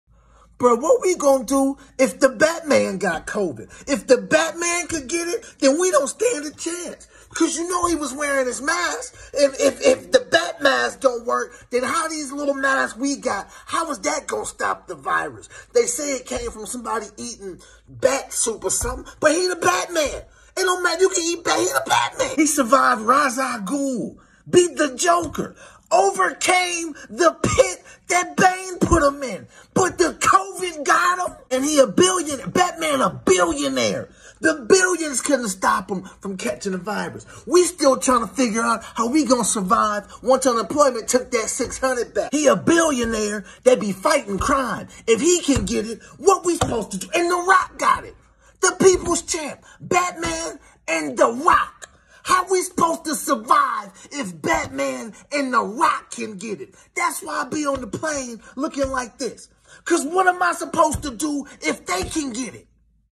Batman has Covid
Robert Pattinson Tests Positive for Coronavirus, Halting ‘Batman’ Production

SirLanceWoods